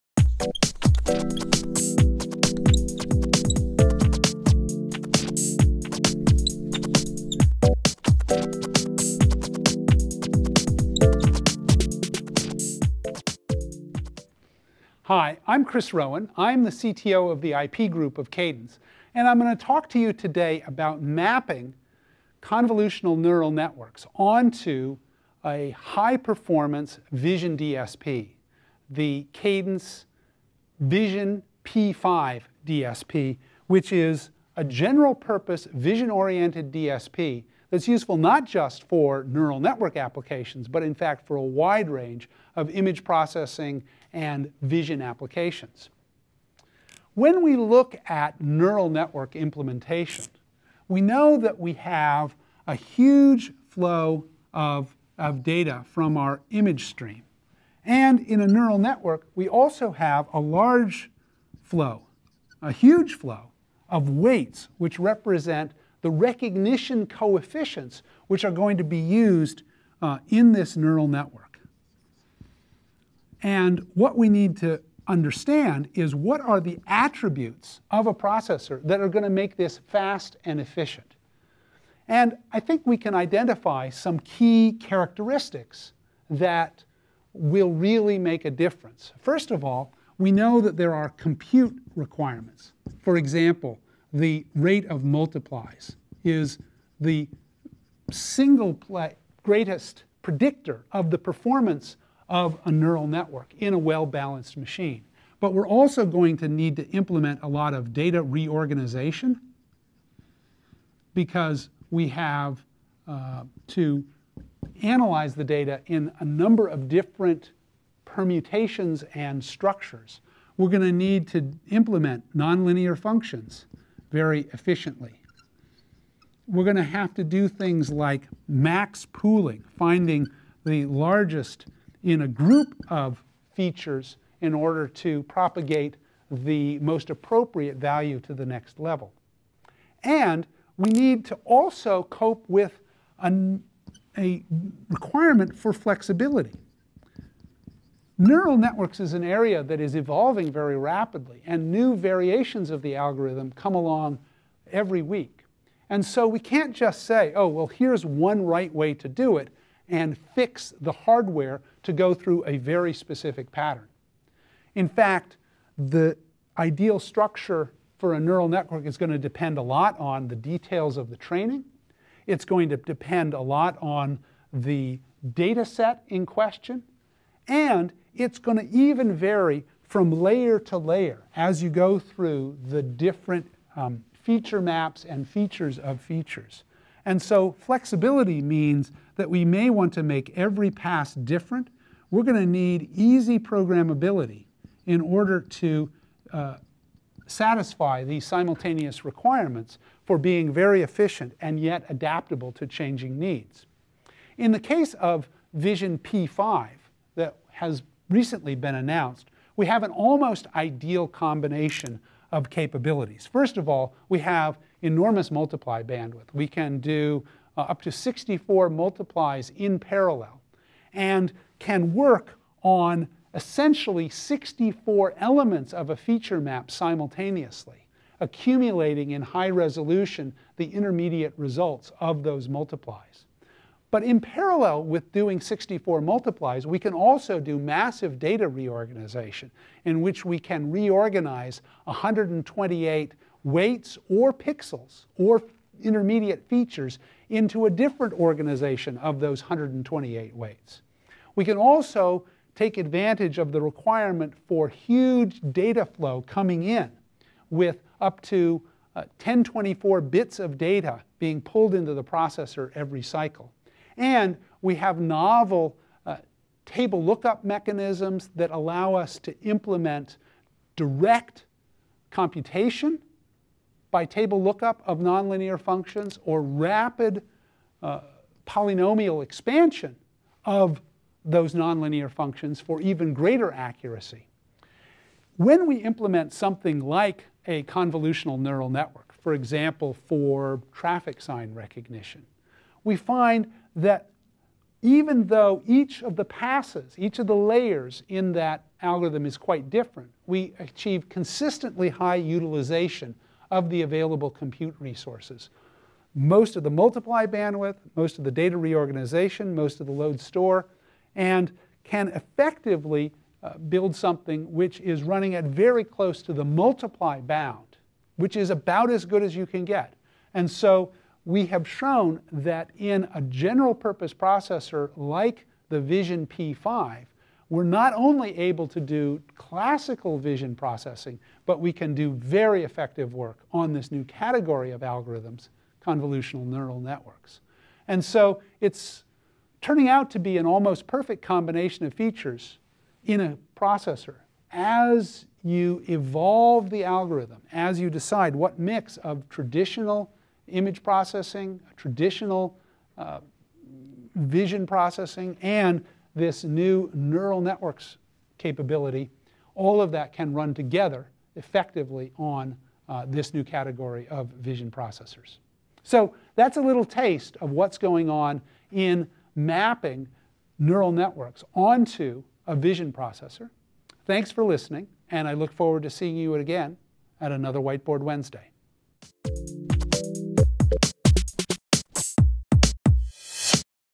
Whiteboard Wednesdays - Mapping Convolutional Neural Networks to the Vision P5 DSP
In this week's Whiteboard Wednesday, Chris Rowen explains how convolutional neural networks (CNNs) map onto the high-performance Tensilica Vision P5 DSP. He describes the benefits that the programmability brings, giving the flexibility needed for the ongoing development of these algorithms alongside the more traditional image processing and computer vision applications.